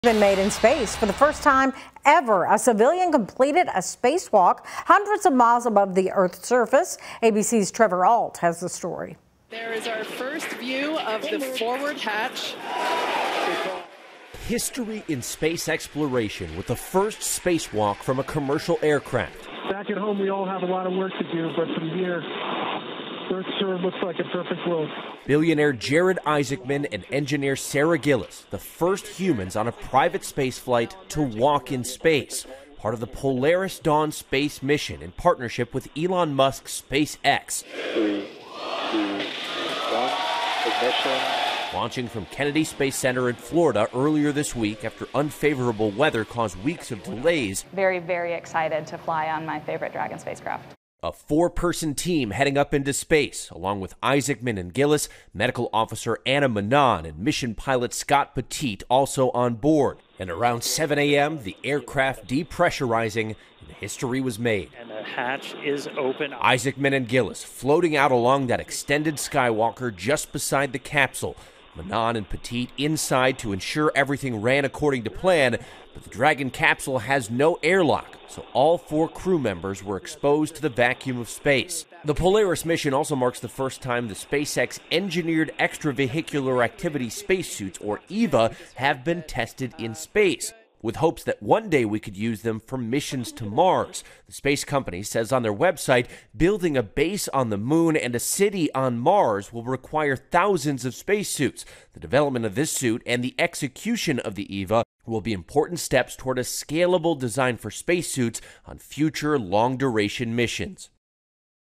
Two people complete historic spacewalk on private space flight
Billionaire Jared Isaacman and engineer Sarah Gillis are the first humans on a private space flight to walk in space.